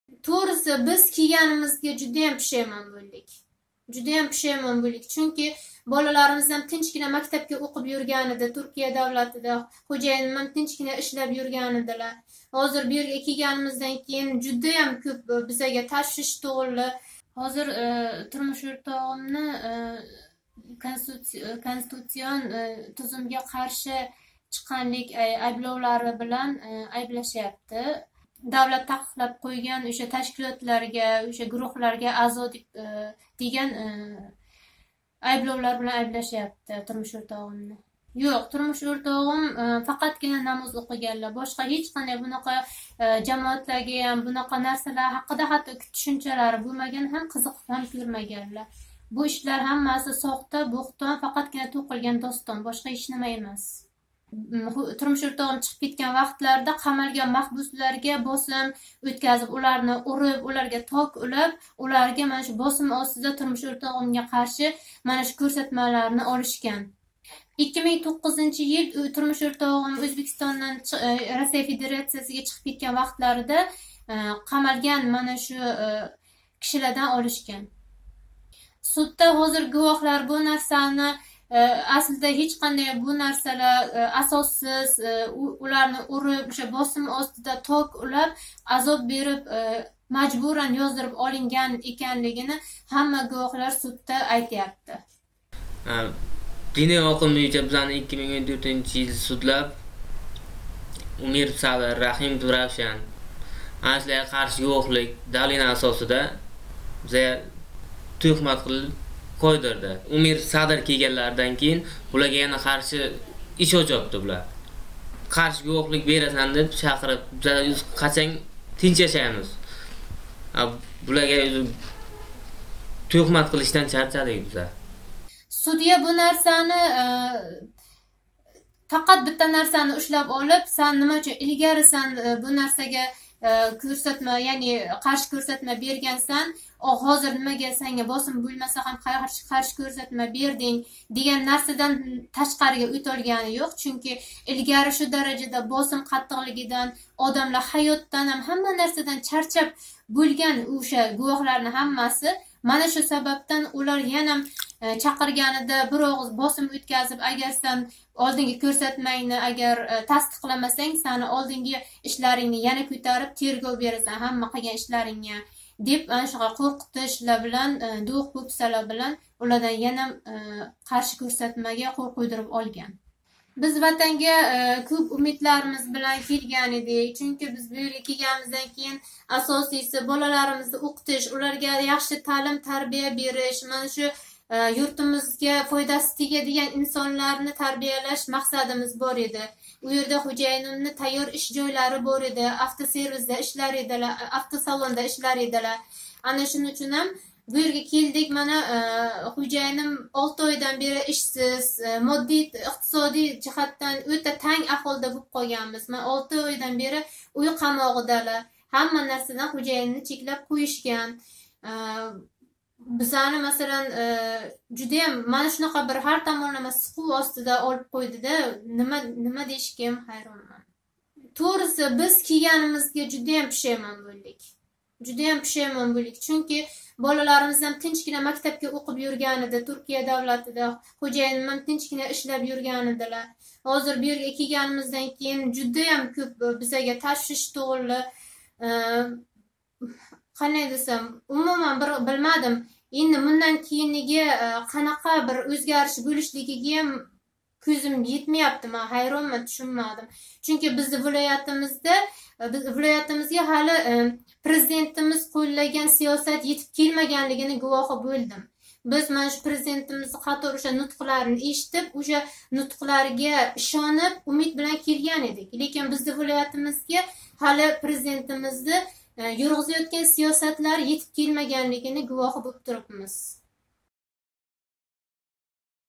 Ўзбекистон: Туркиядан Навоийга қайтганимизга пушаймонмиз - BBC Uzbek
Ўзбекистон: Навоийга Мирзиёев сиёсати етиб бормаган

Туркияда муҳожирликда юрган Умировлар оиласи Ўзбекистонга қайтганидан пушаймон эканлигини айтишмоқда.  Садирбек Умиров оиласи билан 2018 йилнинг июль ойида Президент Мирзиёев ваъдаларига ишониб ортга қайтган. Бироқ Навоий вилоятида уни конституцион тузумга тажовуз қилишда айблашмоқда.

Садирбек Умиров 2009 йили Навоийнинг Кармана туманидаги танишлари қамоққа ташлангач, унга нисбатан туҳмат бўлишидан қўрқиб Ўзбекистондан чиқиб кетишга мажбур бўлган.

"Турмуш ўртоғим чиқиб кетишидан бир неча ой ўтиб, уни танийдиган одамларга қаттиқ қийноқ қўллаб қарши кўрсатмалар ёзиб олишган. Шундан сўнг эрим Ўзбекистонга келмади. Бизни ҳам 2015 йилда ўзи яшаётган Туркияга чақириб олди", дейди унинг рафиқаси Дилрабо Умирова.

Дилрабо Умированинг айтишича, Президент Шавкат Мирзиёевнинг ваъдалаоидан сўнг, улар билан Навоий вилоятидаги ички ишлар органи ходимлари боғланиб Ўзбекистонга қайтаверишини ва муаммо бўлмаслигини айтишган.

Бироқ улар ортга қайтганидан сўнг, Садирбек Умировга нисбатан қўзғатилган жиноий иш давом эттирилади.

38 ёшли Садирбек Умиров уй қамоғига ҳукм қилинган ва унинг суди давом этмоқда.

"Эримга нисбатан қўйилган айб, қуруқ туҳмат. Айблар 2009 йили гувоҳларни электр токи каби даҳшатли нарсалар билан қинашганидан сўнг қўйилган. Судда иштирок этган гувоҳлар ўша вақти қийноқларга тутилганини ва мажбур бўлганини айтишди, бироқ судя нимага бу сафар ҳам кўрсатмаларга қўл қўйиб бериб, энди судда бошқача гапиряпсизлар, деб инобатга олмаяпти", дейди Дилрабо Умирова.

Унинг айтишича, гувоҳлар судьяга бу сафар ҳам ички ишлар органи ходимлари томонидан оғзаки равишда қўрқитилганидан сўнг, ушбу кўрсатмаларга қўл қўйиб берганини айтишган.

Гувоҳлардан бири Достон Ҳалимов Би-би-сига 2014 йилда қийноқлар остида Садирдек Умировга қарши кўрсатма ёзиб беришга мажбур бўлганини айтади.

"Ёлғон кўрсатмалардан чарчадик, бу қачонгача давом этади", дейди у.

Садирбек Умиров 6 ойдан бери уй қамоғида, суд унинг телефонда ёки бошқа воситалар орқали бошқалар билан гаплашишига тақиқ қўйган.

"Хўжайиним Туркияда автасалонда ишлар эди. Ўзбекистонга келиб, 6 ойдан бери ҳеч қаерда ишламайди. Моддий иқтисодий жиҳатдан ўта танг аҳволда бўлиб қолганмиз. 6 ойдан бери уй қамоғида, ҳамма нарсадан хўжайинимни чеклаб қўйишган. Ҳар томонлама сиқув остидамиз".

Дилрабо Умирова ваъдаларга ишониб Ўзбекистонга қайтганидан жуда ҳам пушаймон қилаётганини айтади.

"Тўғриси, биз келганимизга жуда ҳам пушаймон бўлдик. Чунки болаларимиз тинчгина мактабда ўқиб юрган эди. Хўжайиним тинчгина ишлаб юрган эди. Бундан кейин қандайдир ўзгариш бўлишига кўзим етмаяпти. Бизнинг вилоятимизга Президент қўллаган сиёсат етиб келмаганини гувоҳи бўлдим. Президентнинг нутқларини эшитиб, умид билан келган эдик", дейди Навоий вилоятидан Дилрабо Умирова.